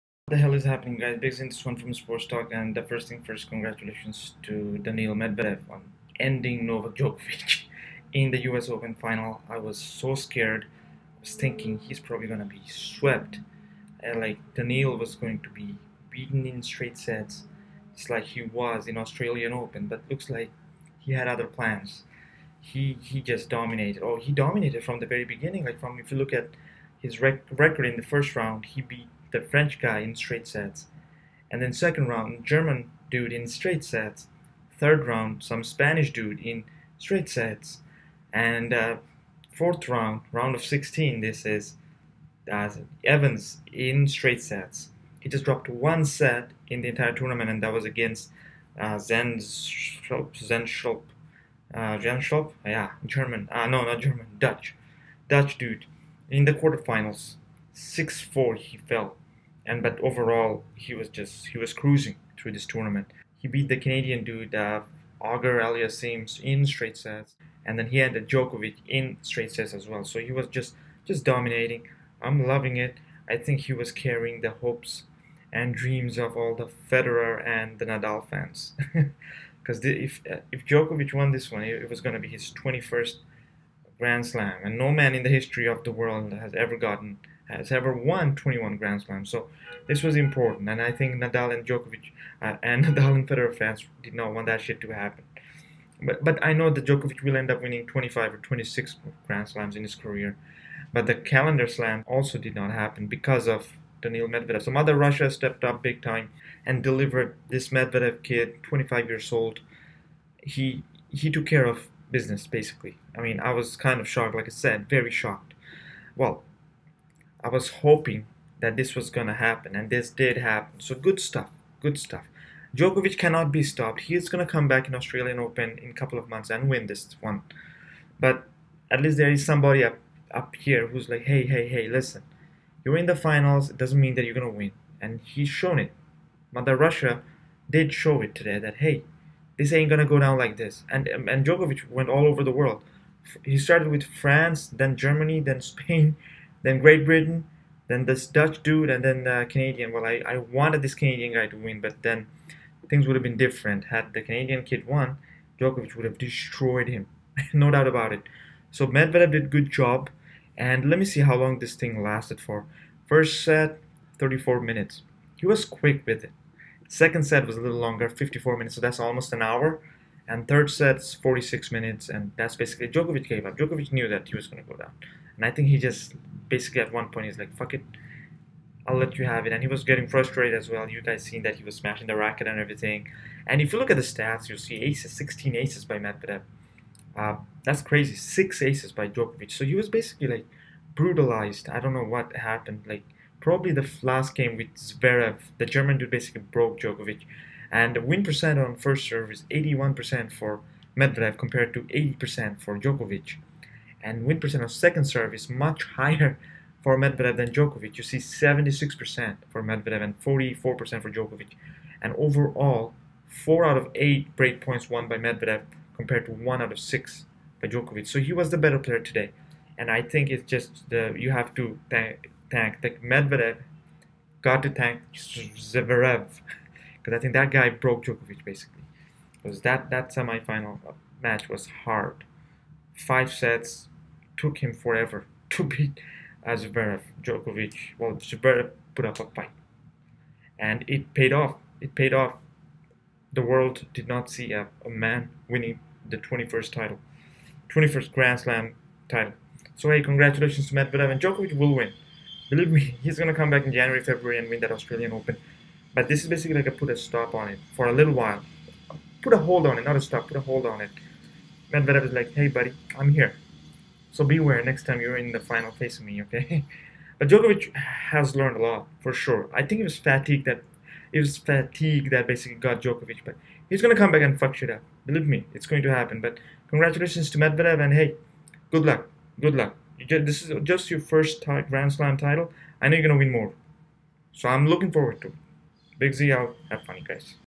TENNIS REACTION - FIFA GAMER MEDVEDEV ENDS DJOKOVIC'S DREAM OF WINNING 21st GRAND SLAM
Boy am I happy to see Daniil Medvedev winning this one against Novak Djokovic😃
In no way am I a Djokovic hater,  but I am just glad that DJOKOVIC has received some sort of competition from someone in the Tennis world. God Bless MEDVEDEV and God Bless Mother Russia😂